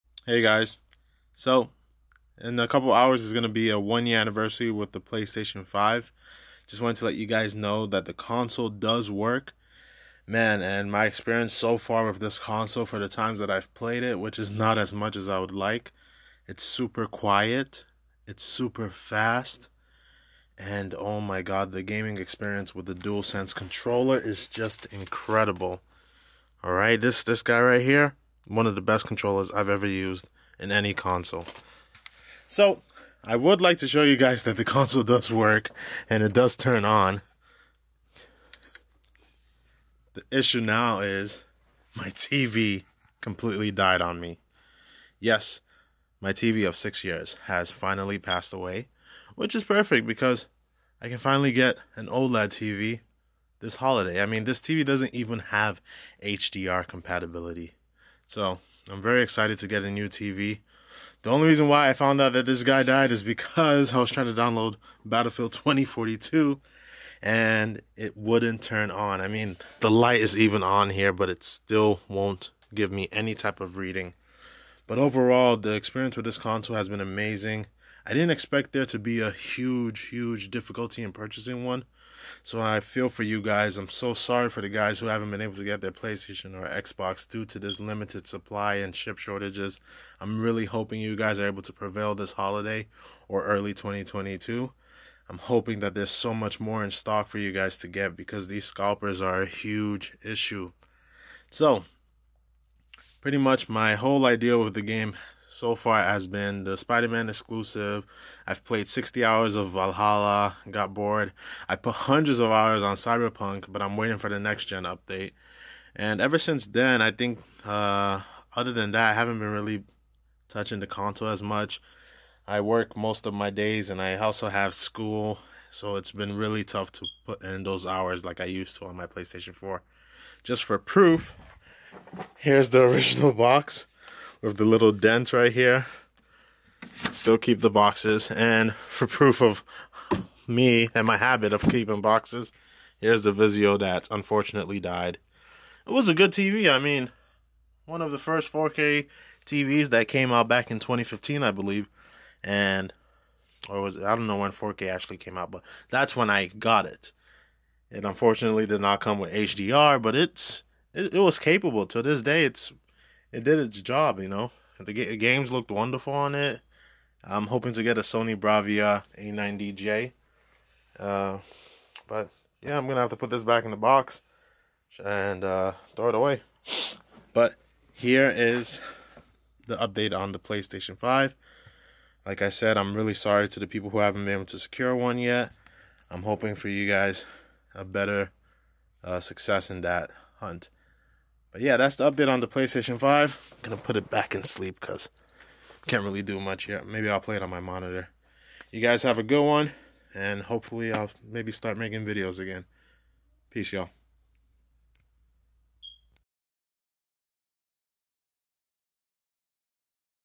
Update on my Playstation 5
Happy Playstation 5 anniversary just wanted to give you guys an update.

TV died so ill be using a monitor until I get that OLED TV